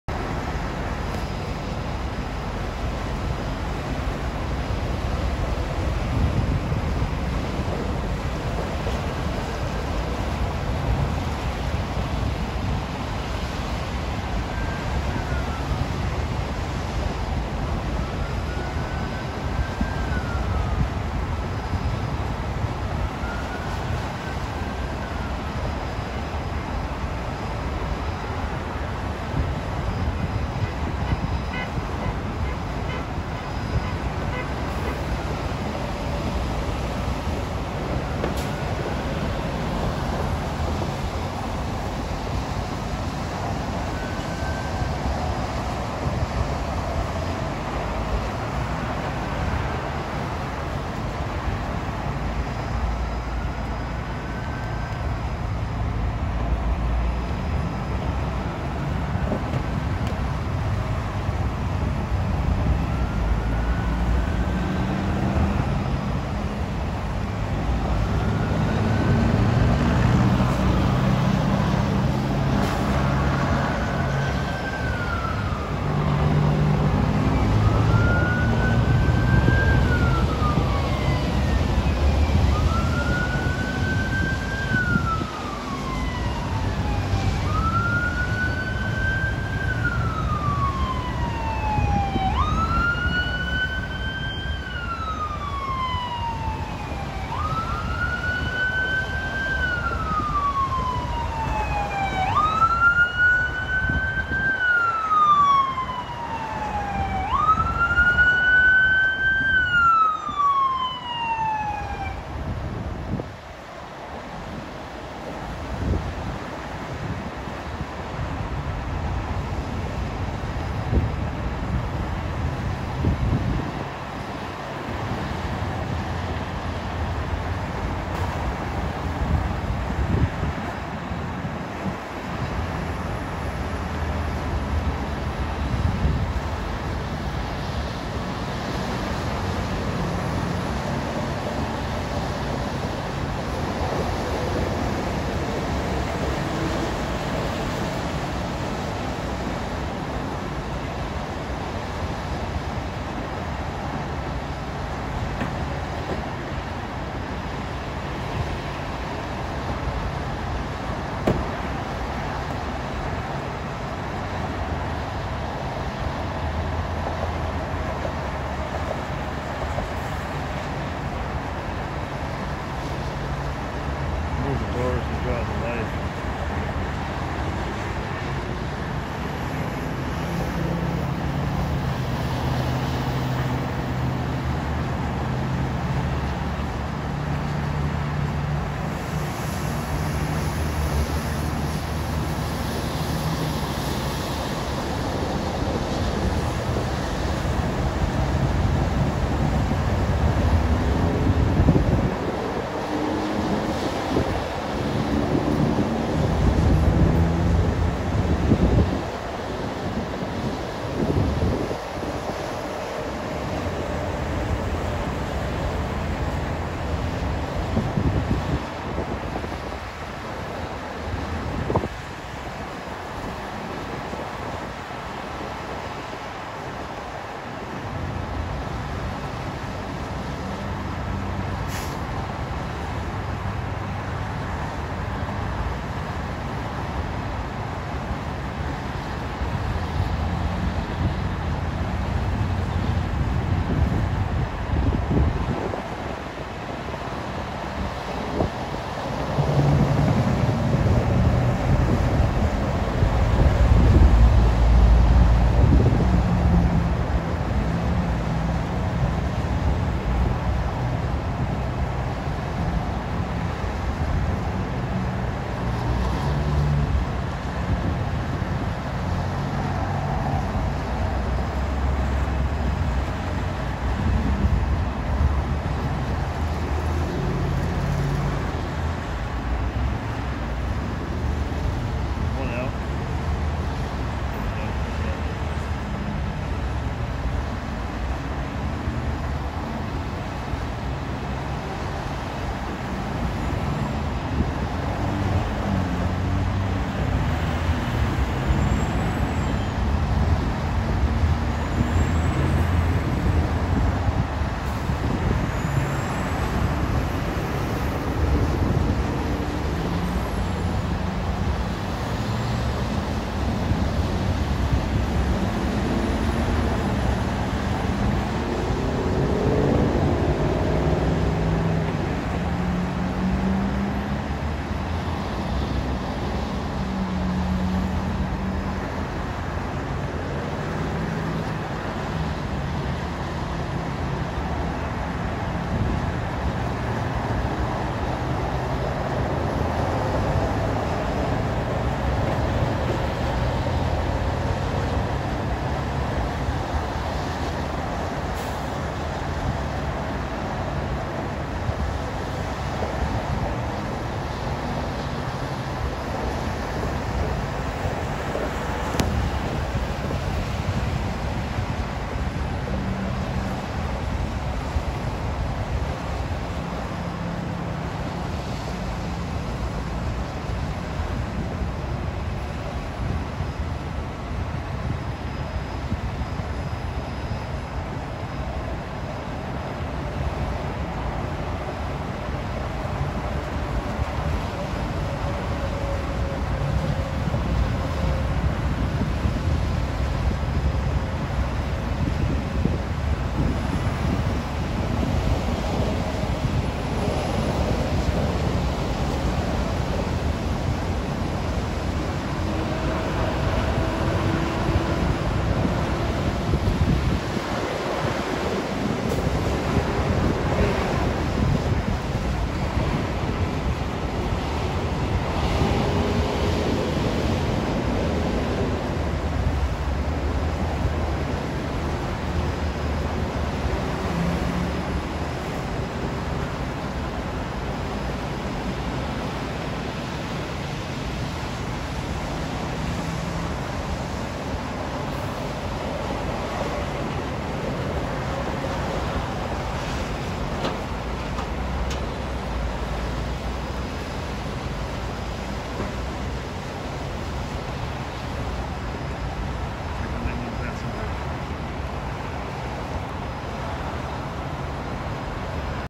WATCH NOW: Firefighters extricate driver after rollover accident
Buffalo firefighters appear to have succeeded in removing an individual from a vehicle that flipped over as it approached the I-190's Elm Street southbound exit at rush hour Wednesday morning in downtown